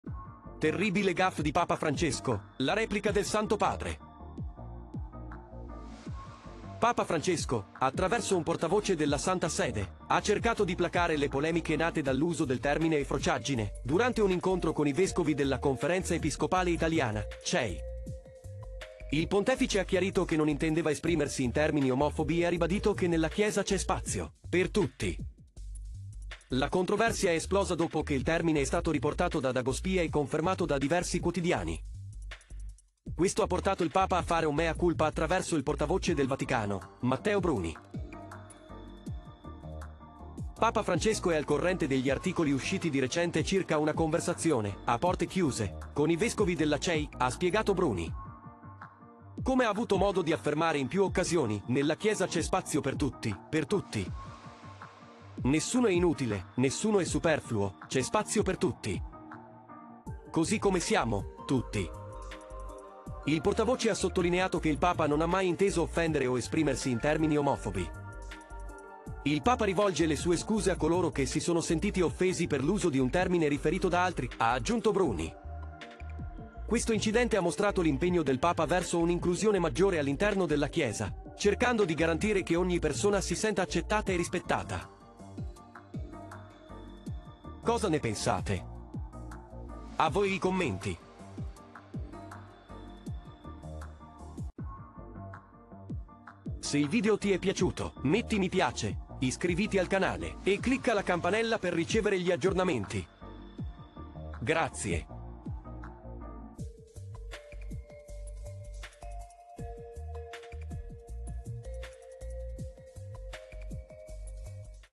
Terribile Gaffe Di Papa Francesco: La Replica Del Santo Padre!
Papa Francesco ha recentemente fatto una terribile gaffe durante un incontro episcopale, che ha scatenato tutti i social. Il Papa ha finalmente risposto. Ecco che cosa ha detto!